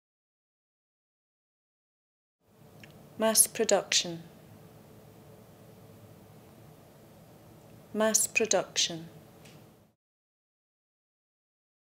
How to pronounce MASS PRODUCTION in British English
This video shows you how to pronounce MASS PRODUCTION in British English. Speaker has an accent from Lanarkshire, Scotland. Collins Dictionary, the home of living English and pioneers of dictionary publishing